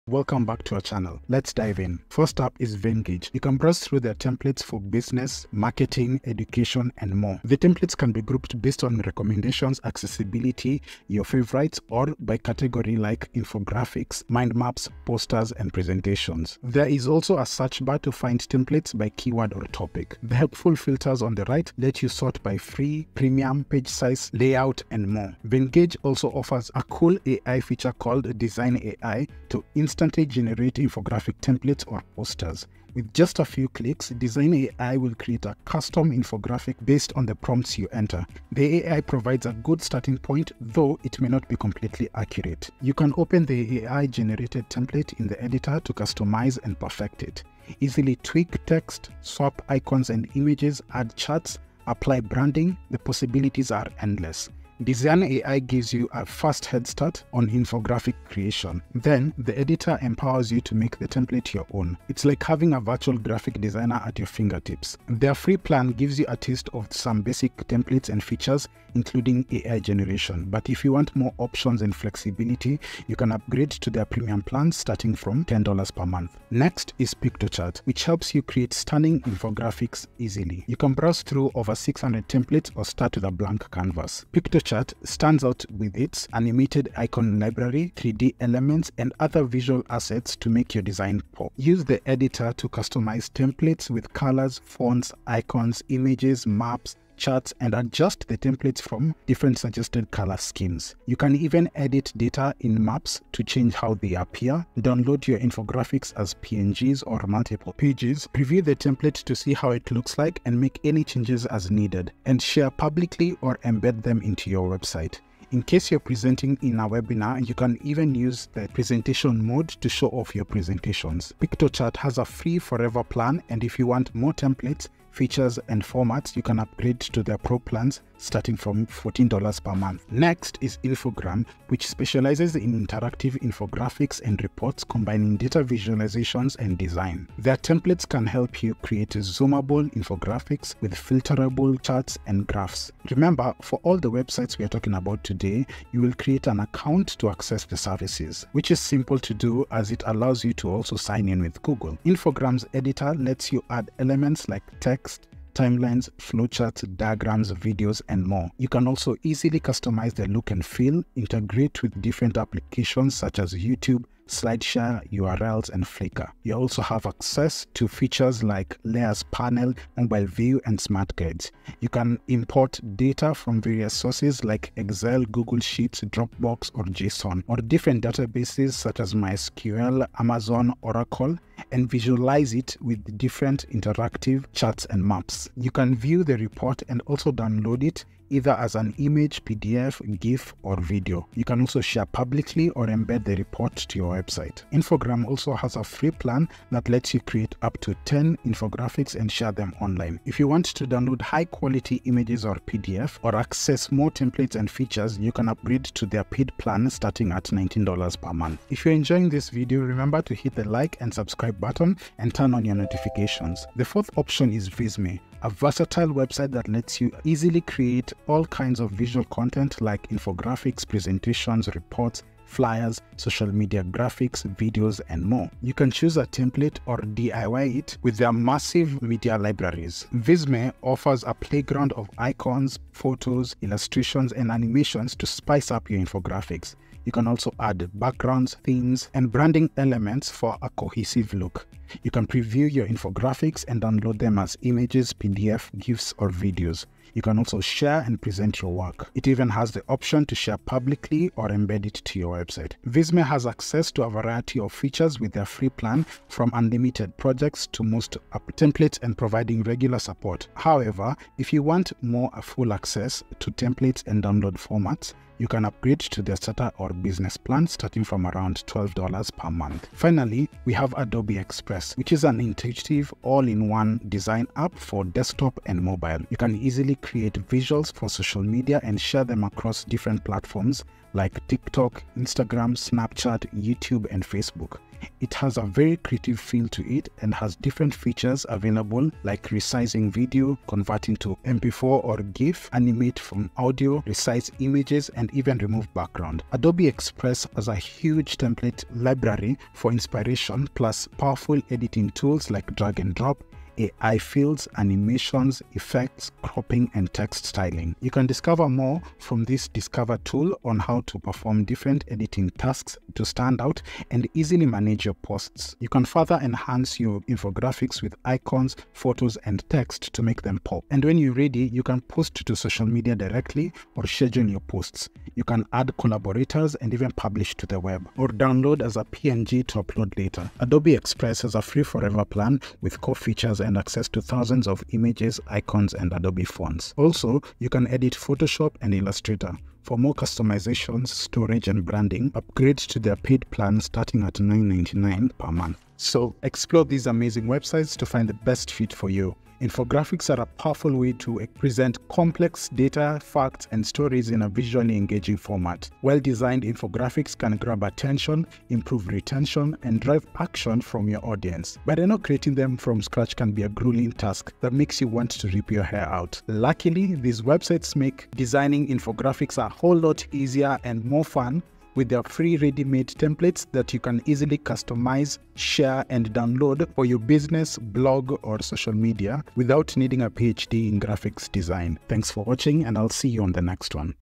AI-Powered Infographic Makers
In today's data-driven world, infographics have become an indispensable tool for communicating complex information in a visually engaging and easily understandable way. With the rise of artificial intelligence (AI), infographic creation has reached new heights of efficiency and creativity. In this video, we explore the top 5 AI-powered infographic makers: Venngage, Piktochart, Infogram, Visme, and Adobe Express. Their AI-powered features include Smart Data Visualization, AI Text Generatoration, AutoBrand and Smart Themes, Smart Fill and Smart Resize.

⌚Timestamp
0:00 – Venngage
1:15 – Piktochart
2:09 – Infogram
3:33 – Visme
4:29 – Adobe Express
5:45 – Final Thoughts

These AI-powered infographic makers have revolutionized the way infographics are created, making them more accessible, efficient, and impactful. Whether you're a seasoned designer or a complete beginner, these tools have something to offer everyone.